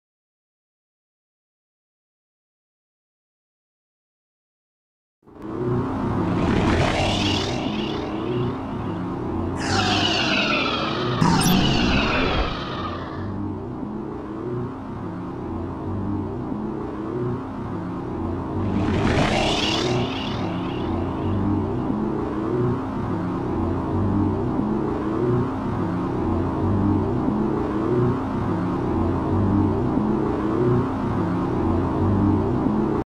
What Happens When Qliphothic Energy Wears Off?
Shrrrriiiiink! :D

I think it's adorable how Elena's aura is bigger than her.